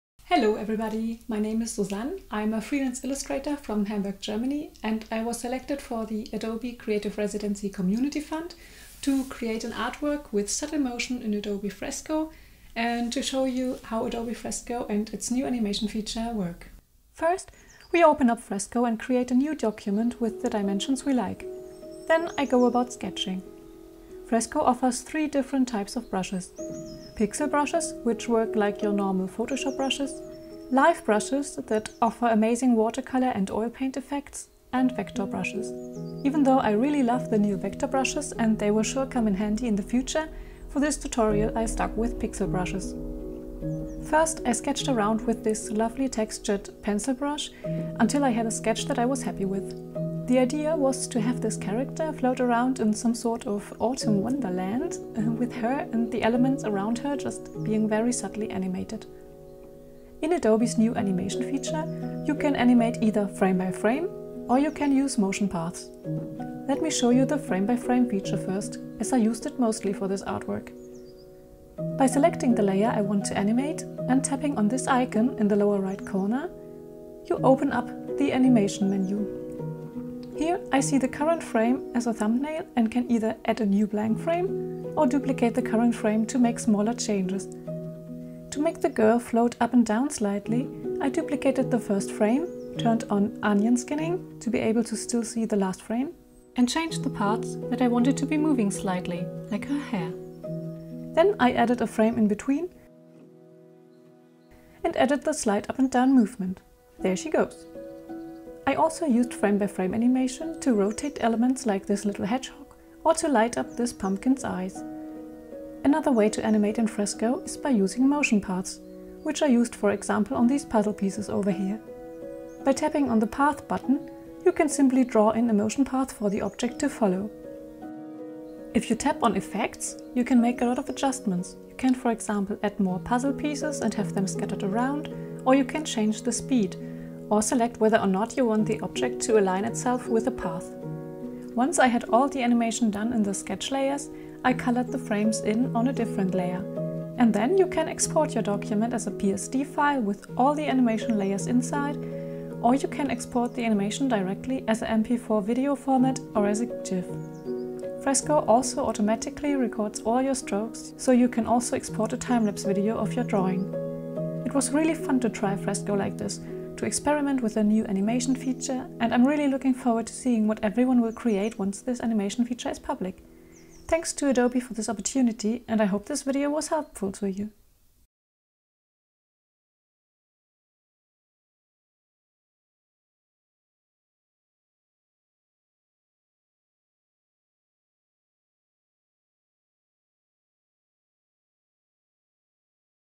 Characters with Subtle Motion in Adobe Fresco | Adobe Creative Cloud
Illustrator and handletering artist Susann Hoffmann shares how to create characters with subtle motion in Adobe Fresco.